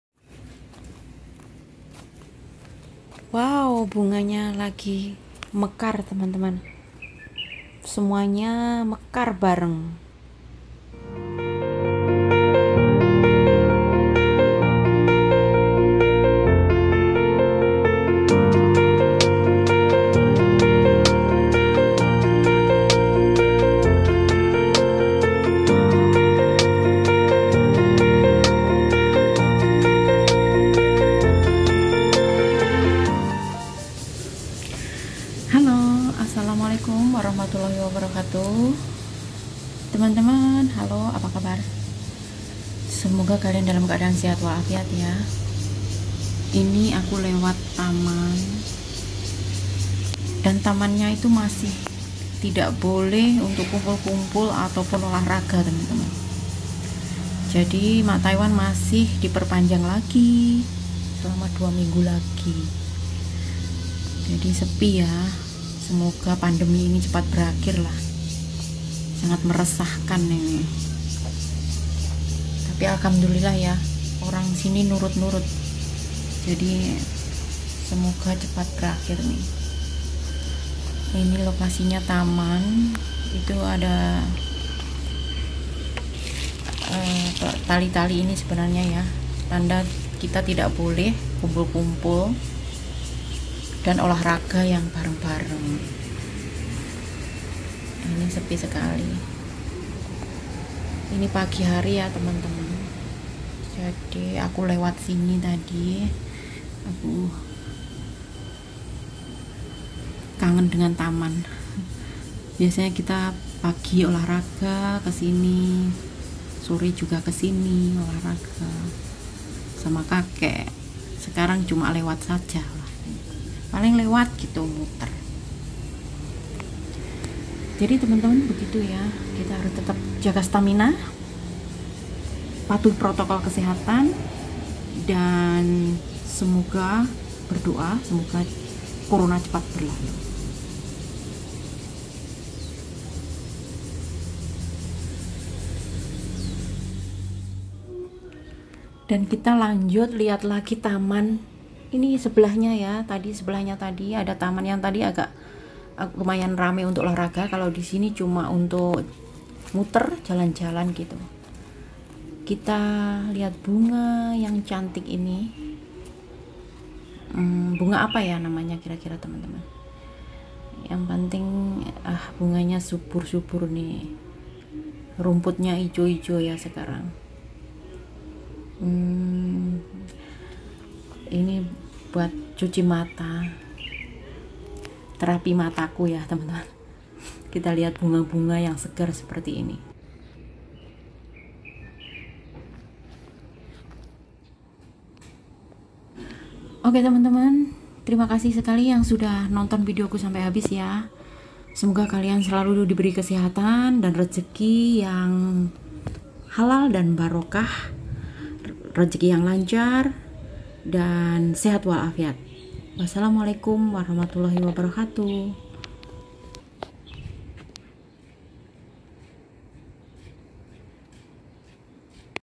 Suasana Taman tempat olah raga Sepi //Bunga2 diTaman
Cerita TKW Taiwan
Taman Olah raga Taiwan
Taman Bunga
Suasana pagi &sore Taman
Waspada Taiwan sudah membaik
Patuhi Protokol Kesehatan
Jaga Lansia Taiwan
Youtuber pemula